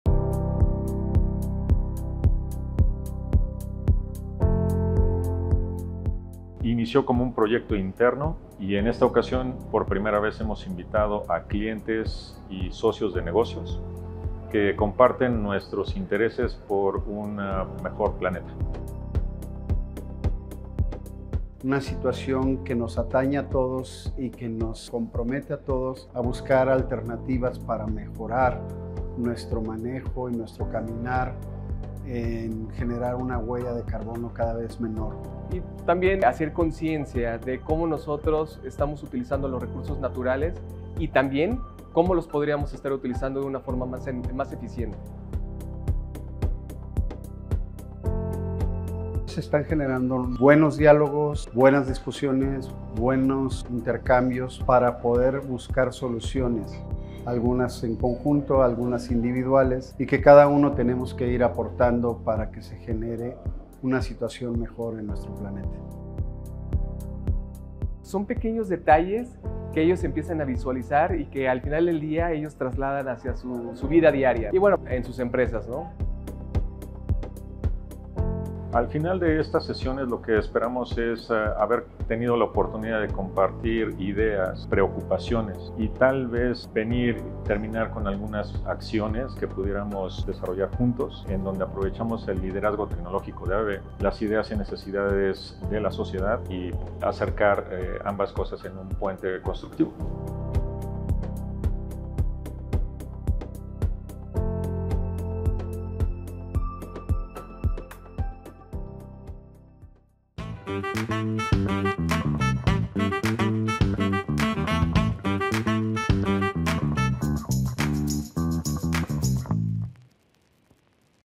Juntos por un planeta más verde: Colaboración y sostenibilidad en Greener in Motion Forum
En este encuentro, reunimos a clientes, socios y expertos para abordar un tema que nos concierne a todos: la sostenibilidad. Descubre cómo estamos trabajando juntos para reducir nuestra huella de carbono y encontrar soluciones innovadoras para un futuro más sostenible. ¡Únete a la conversación y descubre cómo puedes hacer la diferencia!